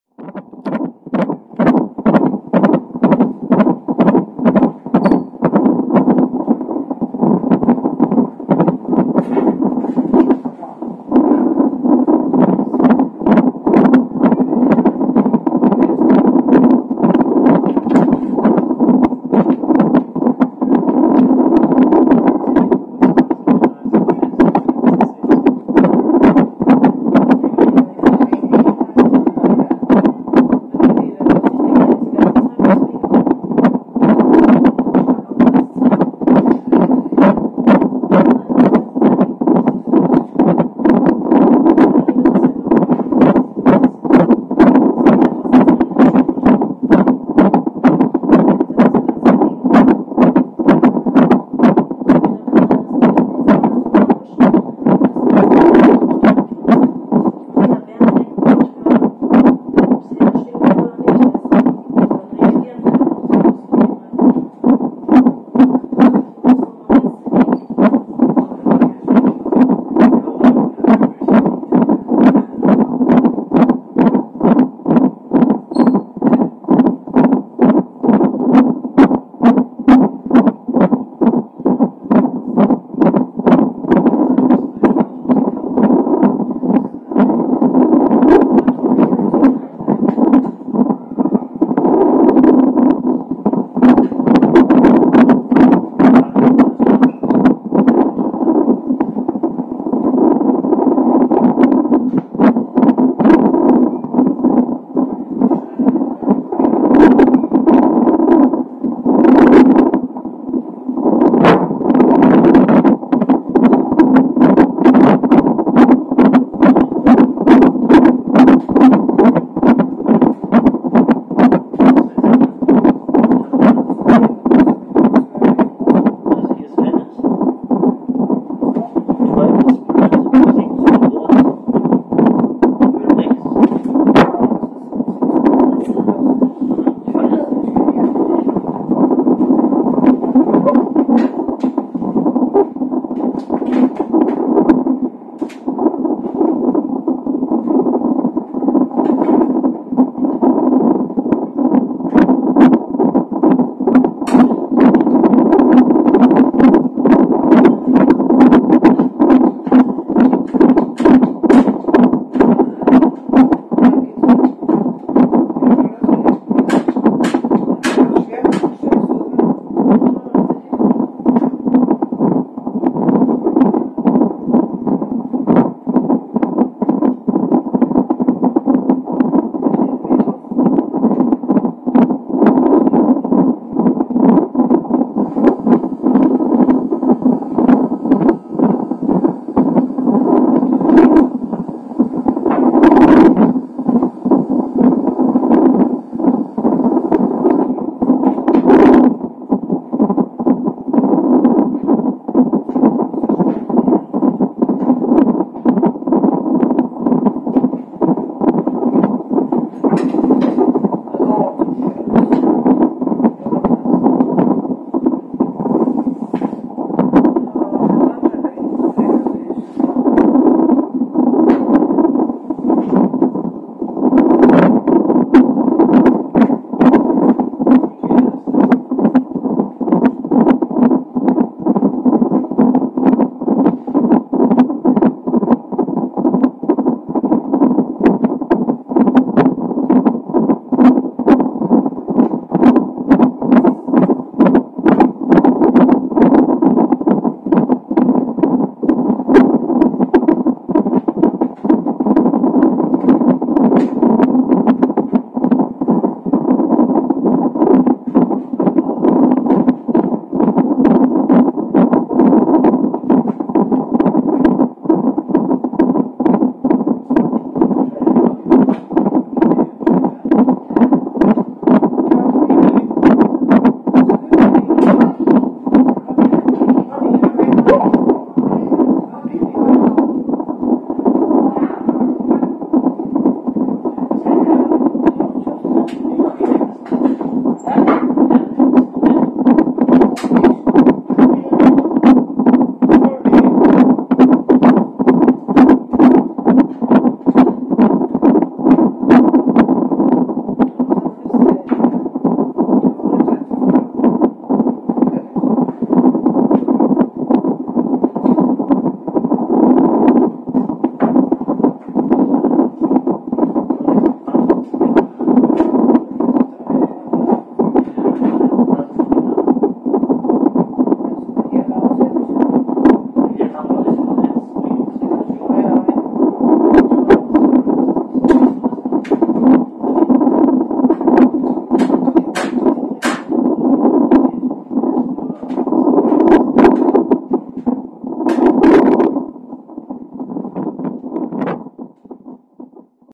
Down-Syndrom Baby - Herztöne / Heart
Wenige Stunden vor der Geburt sind diese Töne endstanden.